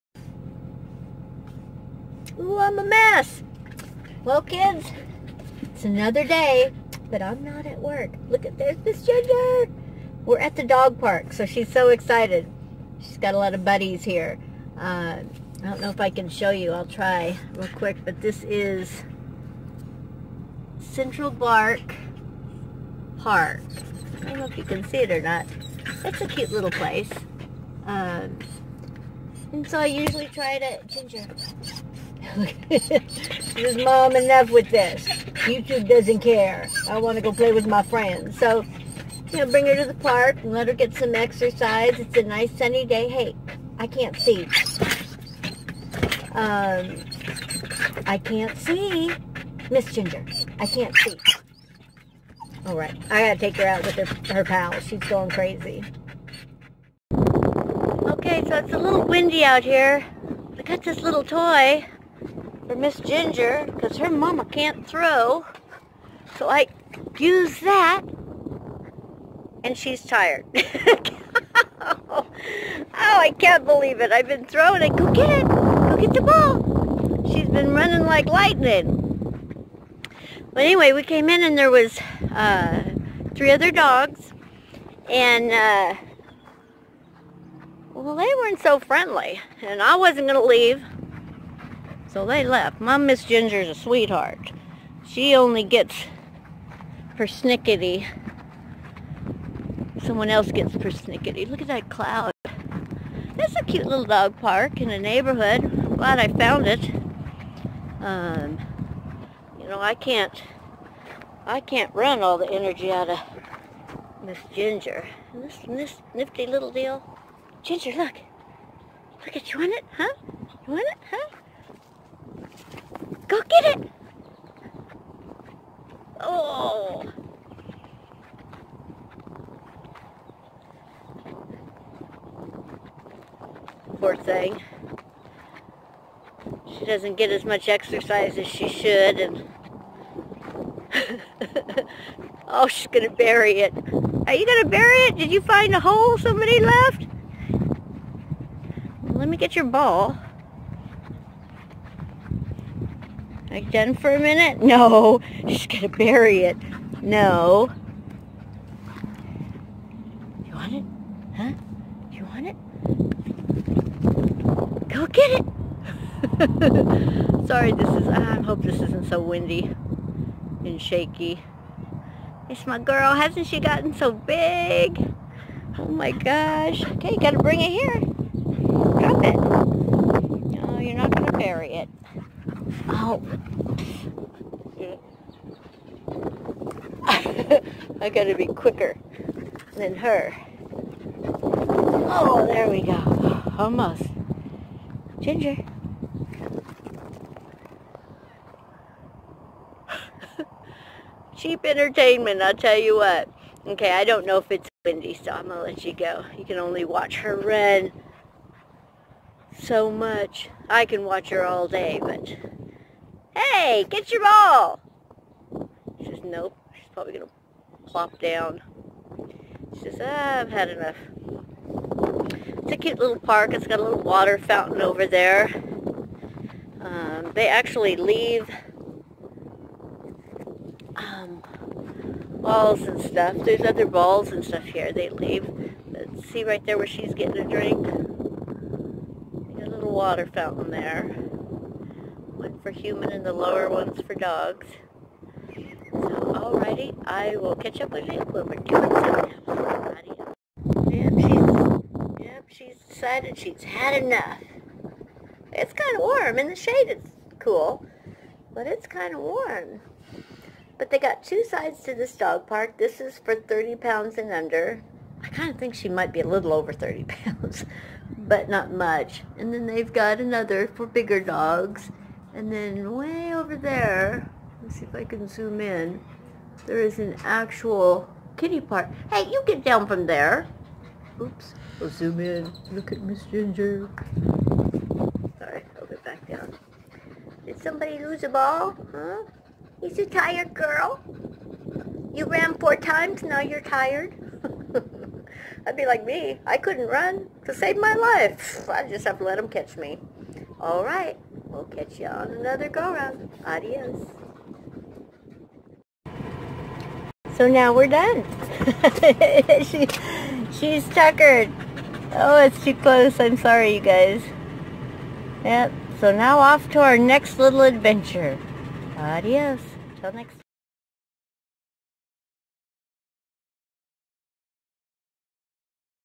Miss Ginger day out ♡♡♡
I'm so glad I found this dog park in Odessa!!!

Gdyup & Get involved let's go camping!!!

GdyUpGal
Dickinson ND
58602

1-855-5GALLOP

I would love your advise on how to better your experience on my channel & advise on video equipment that might work for me...Thanks Y'all :*D